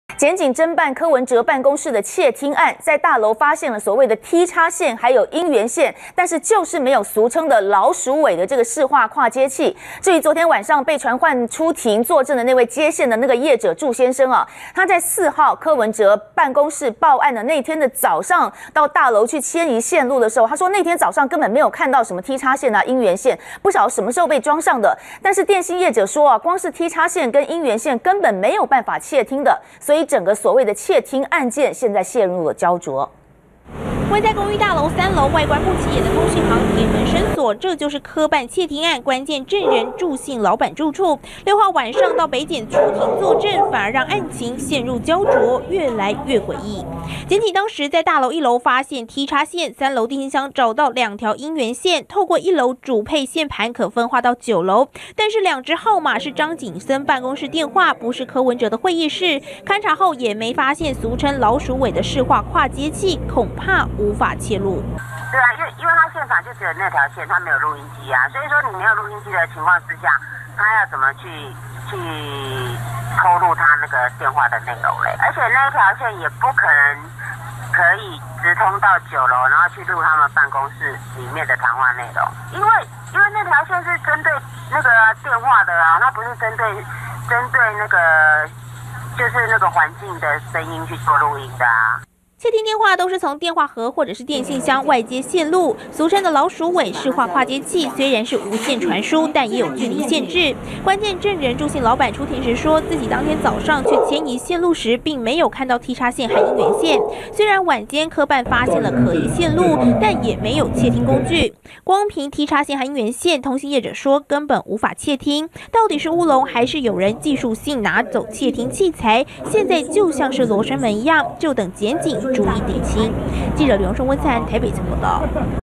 20141107中天新聞 詭異！ 僅T插線、音源線 恐無法竊聽
檢警偵辦柯辦竊聽案，在大樓發現T插線和音源線卻沒有俗稱老鼠尾的市話跨接器，至於6號晚間被傳喚出庭作證的祝先生，在4號柯辦報案當天早上到大樓遷移線路根本沒看到T插線和音源線，不知道是什麼時候被裝上，但是電信業者表示光是T插線和音源線根本沒有辦法竊聽，整起案件陷入膠著。